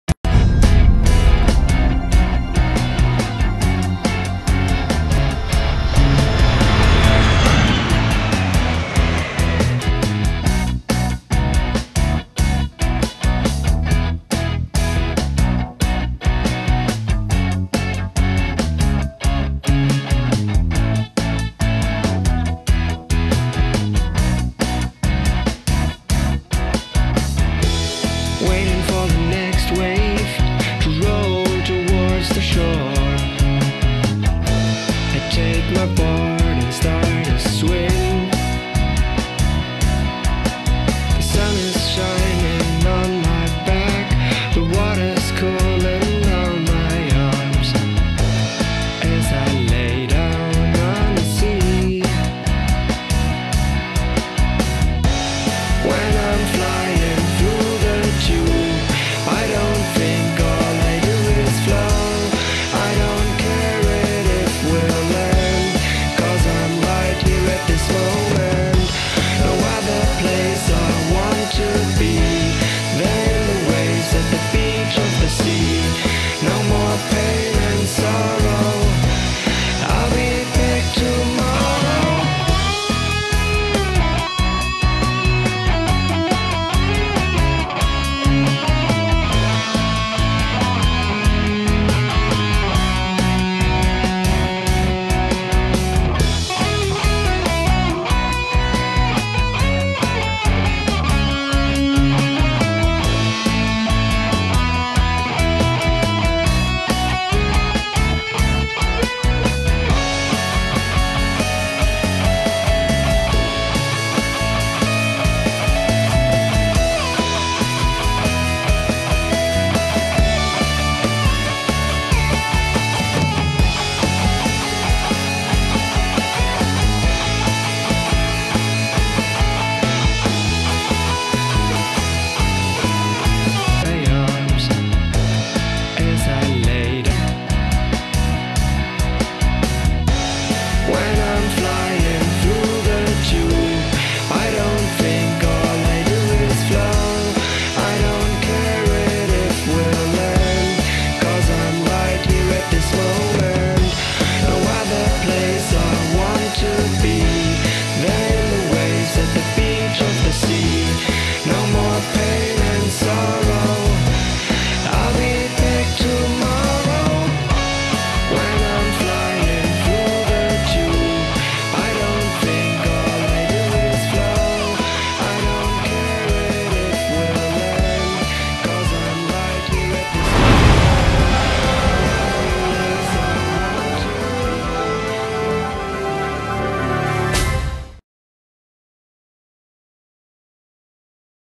portugal
Eine Tour entlang der Westküste der Algarve.